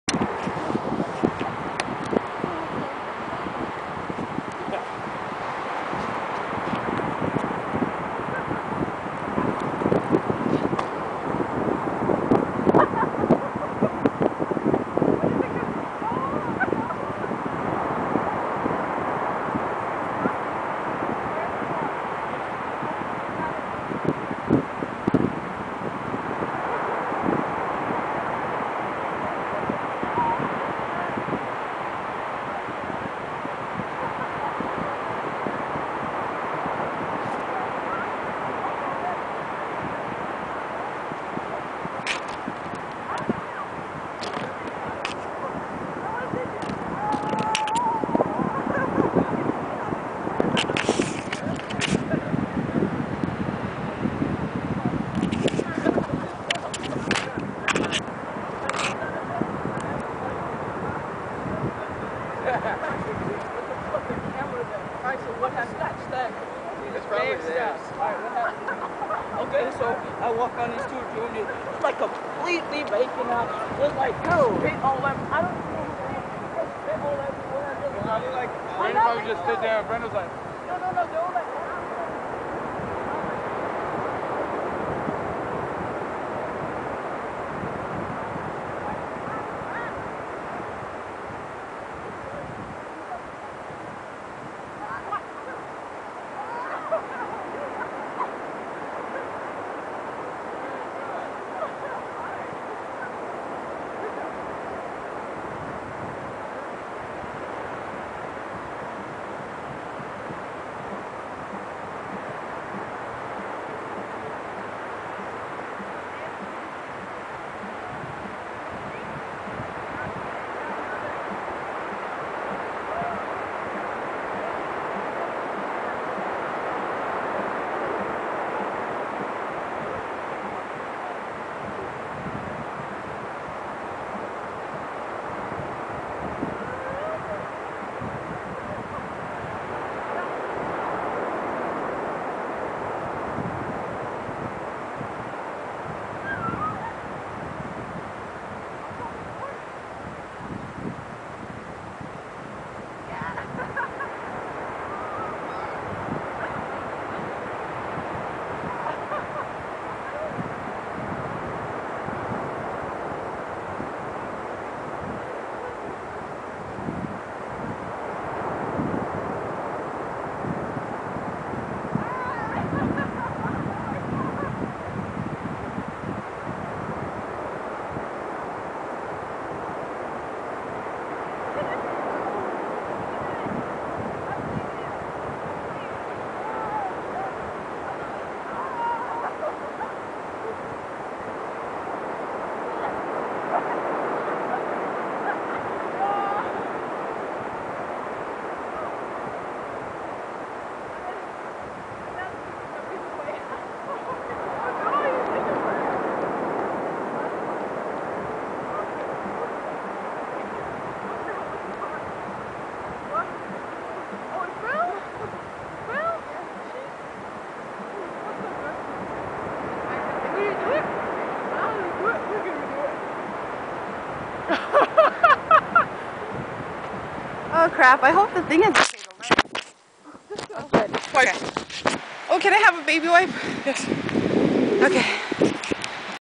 Lindy on the Beach
JJ and Nissreen dance on the shores of NH,